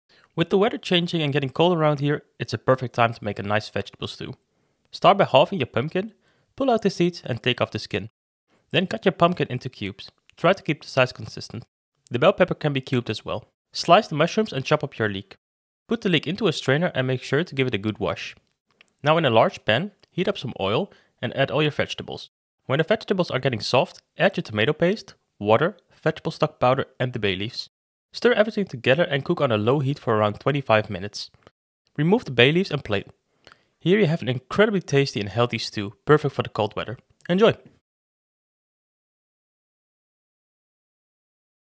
How To Make Vegetable Stew Without any Meat or Dairy
This vegetable stew is a perfect meal for a colder day. It contains pumpkin, leek and other healthy ingredients. it’s super healthy and tasty!